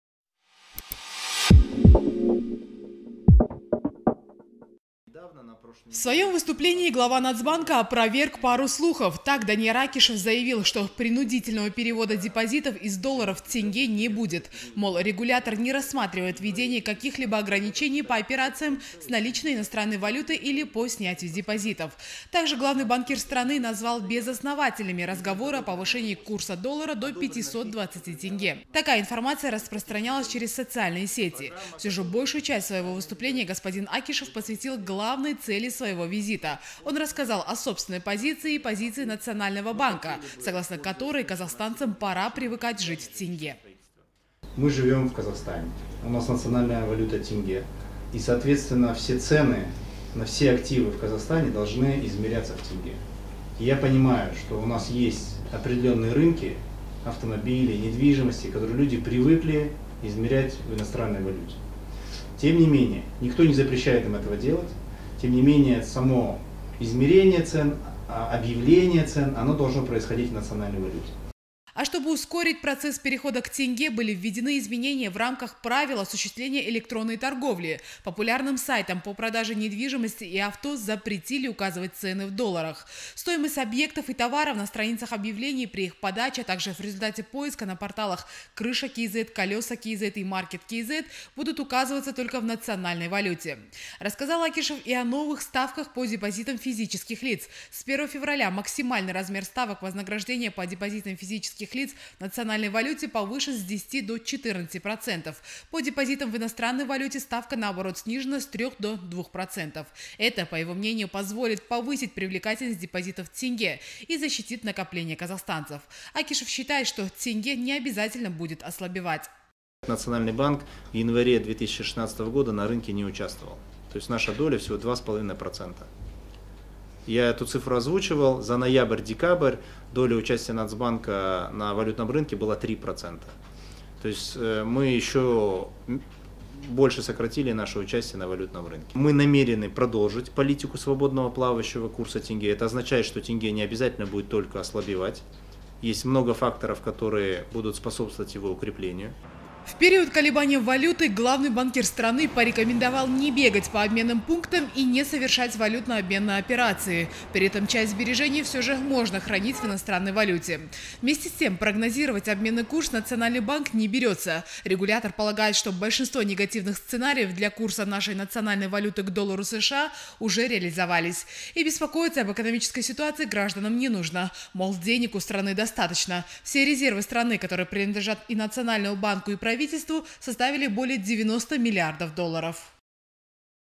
Данияр Акишев: "Мы намерены продолжить политику свободно плавающего курса тенге"
Председатель Нацбанка не может делать долгосрочные прогнозы по поводу обменного курса. Об этом заявил сам Данияр Акишев, находясь с рабочей поездкой в Шымкенте. Он заверил, что самый худший сценарий для нашего курса нацвалюты по отношению к доллару США уже реализовался. При этом, Акишев рекомендовал не бегать по обменным пунктам и не совершать валютно-обменные операции и привыкать жить в тенге.